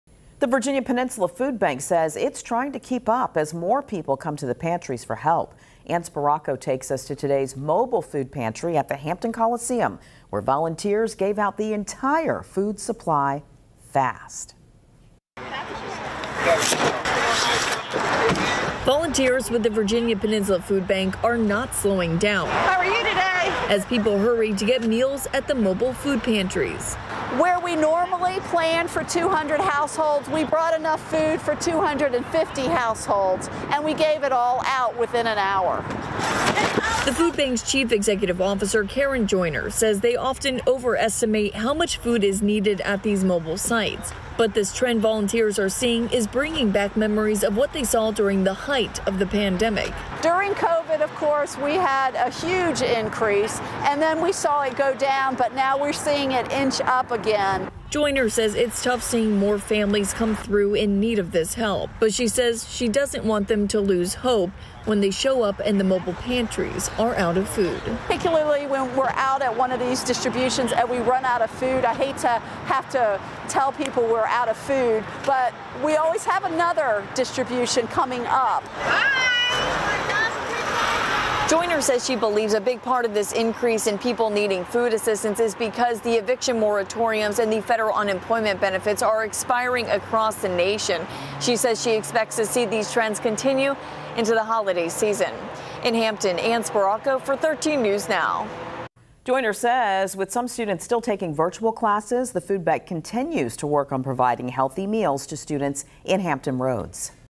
Virginia Peninsula Foodbank holds mobile food pantry
Volunteers hustled around to fill cars with boxes of fresh food and meals for families in the parking lot of the Hampton Coliseum Thursday.